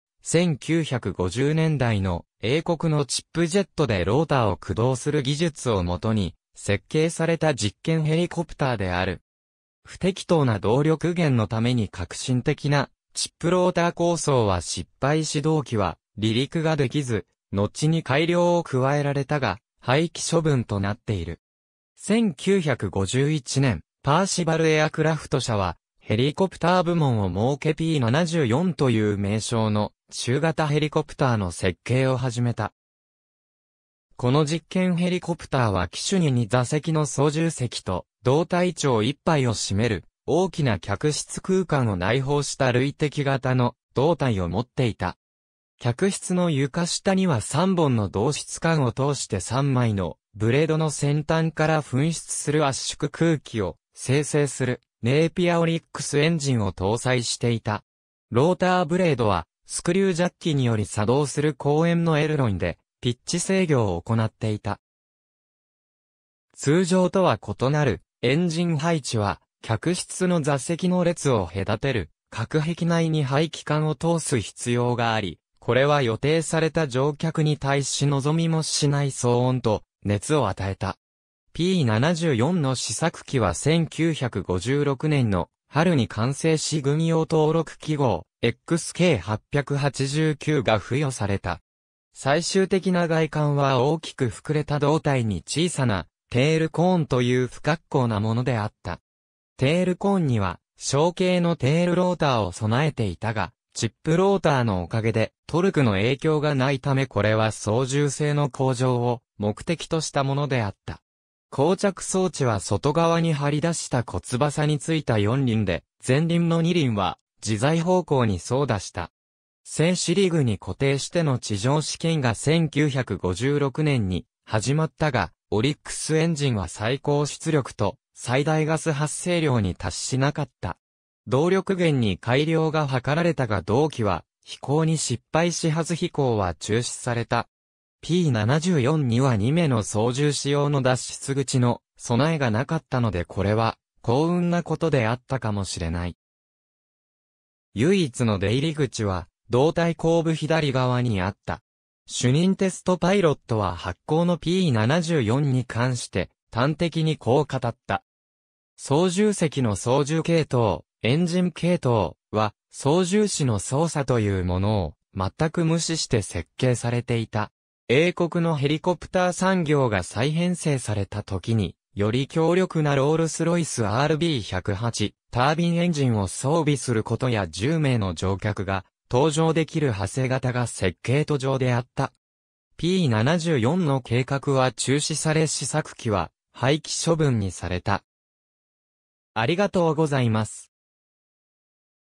パーシヴァル P.74
パーシヴァル P.74 パーシヴァル P.74（Percival P.74、後にHunting Percival P.74）は、1950年代の英国のチップジェットでローターを駆動する技術を基に設計された実験ヘリコプターである。
不適当な動力源のために革新的なチップローター構想は失敗し同機は離陸ができず、のちに改良を加えられたが廃棄処分となっている。
1951年、パーシヴァル・エアクラフト社はヘリコプター部門を設けP.74という名称の中型ヘリコプターの設計を始めた。
この実験ヘリコプターは機首に2座席の操縦席と胴体長一杯を占める大きな客室空間を内包した涙滴型の胴体を持っていた。
客室の床下には3本の導出管を通して3枚のブレードの先端から噴出する圧縮空気を生成するネイピア オリックス（Oryx） エンジンを搭載していた。
ローターブレードはスクリュージャッキにより作動する後縁のエルロンでピッチ制御を行っていた。
通常とは異なるエンジン配置は客室の座席の列を隔てる隔壁内に排気管を通す必要があり、これは予定された乗客に対し望みもしない騒音と熱を与えた。
P.74の試作機（1954年に会社が社名を変更した後にハンティング・パーシヴァル P.74と改称）は1956年の春に完成し軍用登録記号XK889が付与された。
最終的な外観は大きく膨れた胴体に小さな"テールコーン"という不恰好なものであった。
テールコーンには小径のテールローターを備えていたがチップローターのお陰でトルクの影響が無いためこれは操縦性の向上を目的とした物であった。
降着装置は外側に張り出した小翼に付いた4輪で、前輪の2輪は自在方向に操舵した。
静止リグに固定しての地上試験が1956年に始まったが、オリックス エンジンは最高出力と最大ガス発生量に達しなかった。
動力源に改良が図られたが同機は飛行に失敗し初飛行は中止された。
P.74には2名の操縦士用の脱出口の備えがなかったのでこれは幸運なことであったかもしれない。
唯一の出入り口は胴体後部左側にあった。
主任テストパイロットは薄幸のP.74に関して端的にこう語った。
「操縦席の操縦系統、エンジン系統・・・は操縦士の操作というものを全く無視して設計されていた。」 英国のヘリコプター産業が再編成されたときに、より強力なロールス・ロイス RB108 タービン エンジンを装備することや10名の乗客が搭乗できる派生型（P.105）が設計途上であった。
P.74の計画は中止され試作機は廃棄処分にされた。